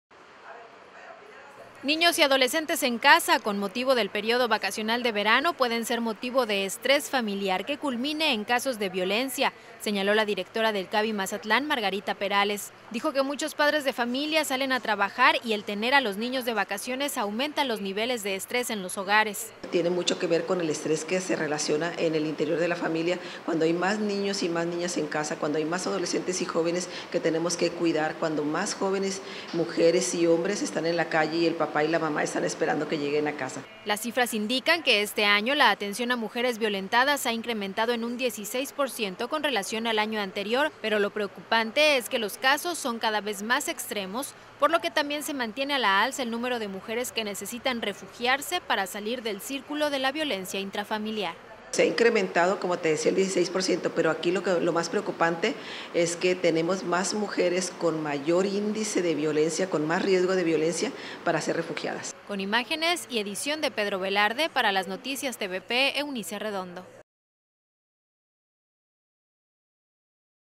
Niños de vacaciones aumentan el estrés familiar
Niños y adolescentes en casa con motivo del periodo vacacional de verano pueden ser motivo de estrés familiar que culmine en casos de violencia, señaló la directora del CAVI Mazatlán, Margarita Perales. Señaló que muchos padres de familia salen a trabajar y el tener a los niños de vacaciones aumenta los niveles de estrés en los hogares.